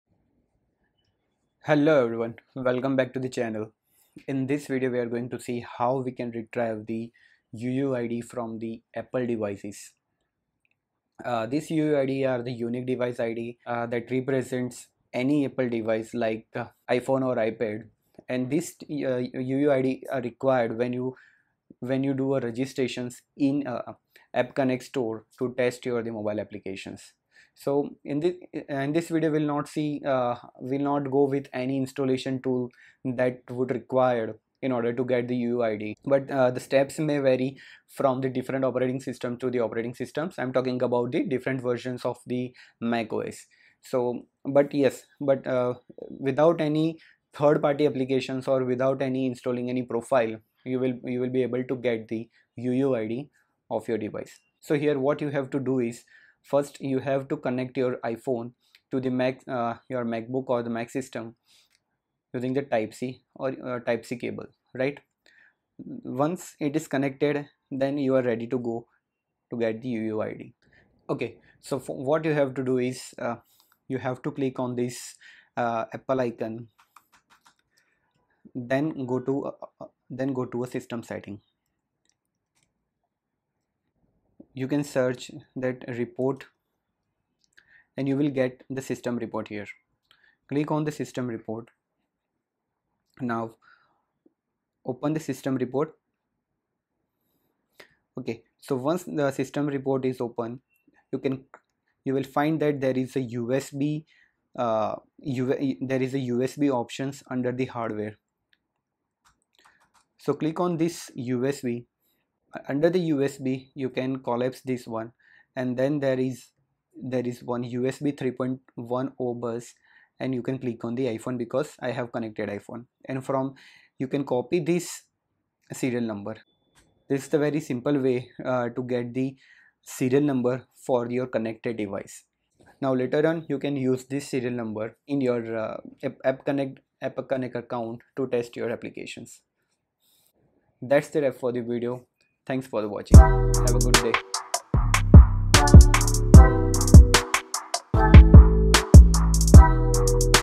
How to get iOS Device's UUID: A Step-by-Step Guide!
Unlock the secrets of your iOS device with simple tutorial on retrieving the Universal Unique Identifier (UUID)! Whether you're a seasoned developer or just curious about the inner workings of your iPhone or iPad, this video will walk you through the process.